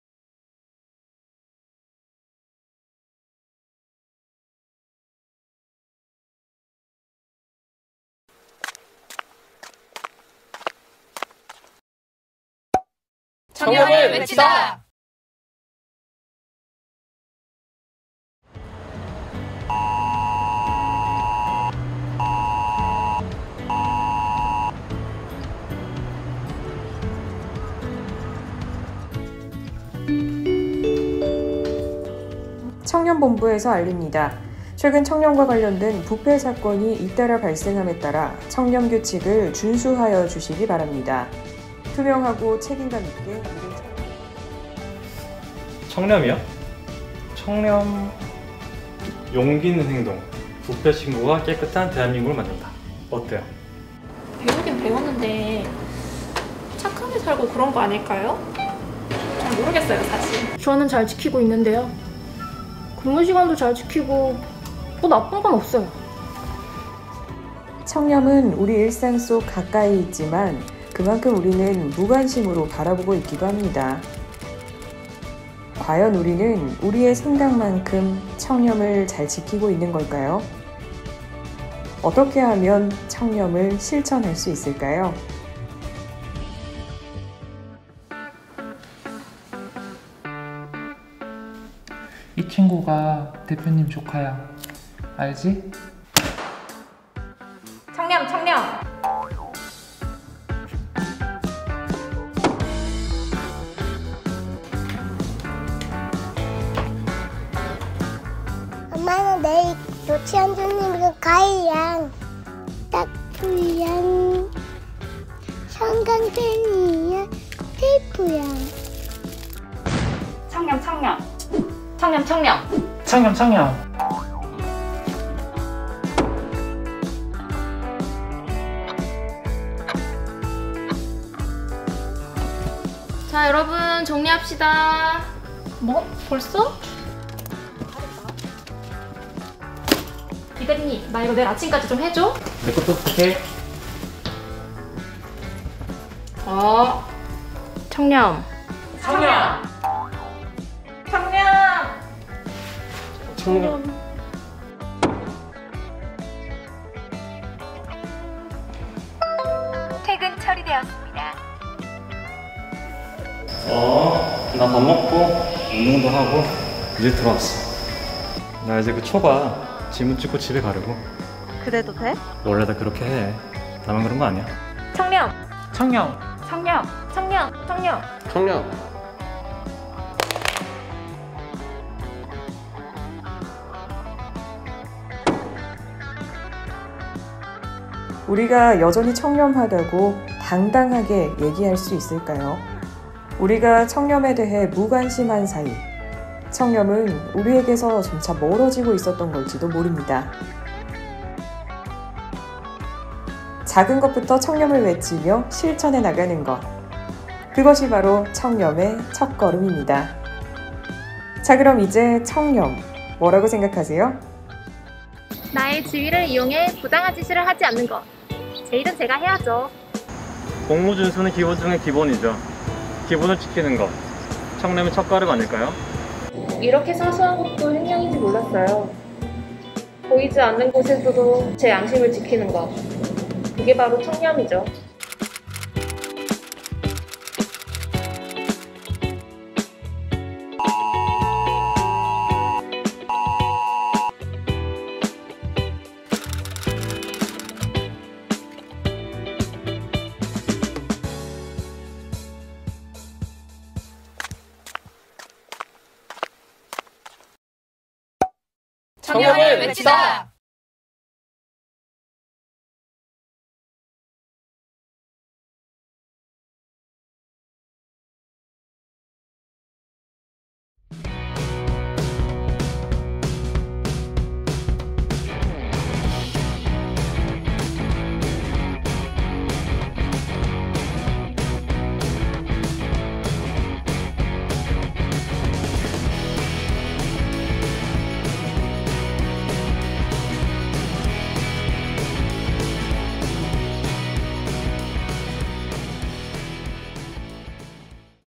[국민권익위 공모전 수상작] 청렴을 외치다
[청렴콘텐츠] 청렴을 외치다

사소한 것부터 지켜나간다면 우리의 청렴은 더 가까워집니다.
2020년 임용된 1년차 공무원들이 직접 제작,출연한 청렴콘텐츠..♥

🎊국민권익위원회 주관
2021년 국민참여 청렴콘텐츠 공모전 장려상 수상작🎊